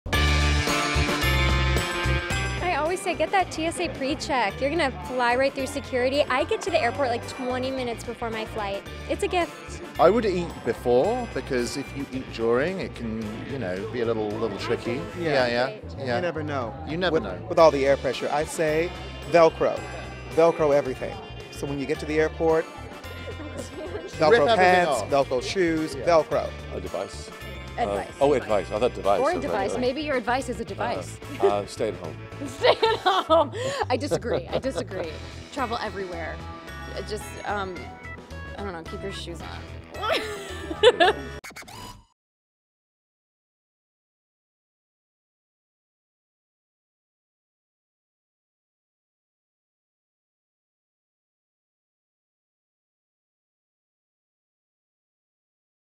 "LA to Vegas" Stars Share Travel Advice | E! Red Carpet & Award Shows
Dylan McDermott, Kim Matula, Olivia Macklin, Ed Weeks and Nathan Lee Graham reveal how to make traveling more pleasurable. Listen!